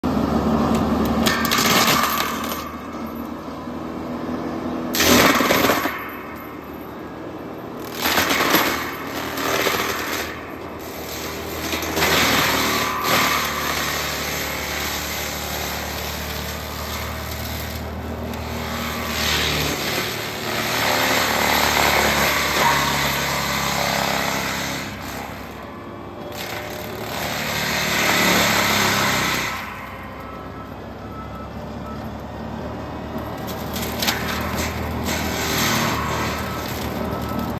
Denis Cimaf 180Di Eating Tree Trunk!
Here is our new John Deere 333G with our Denis Cimaf 180Di attached to it. This combo is such a beast and can handle a lot!

We are located in Des Moines, IA. Give us a call for all your land clearing and dirt work needs!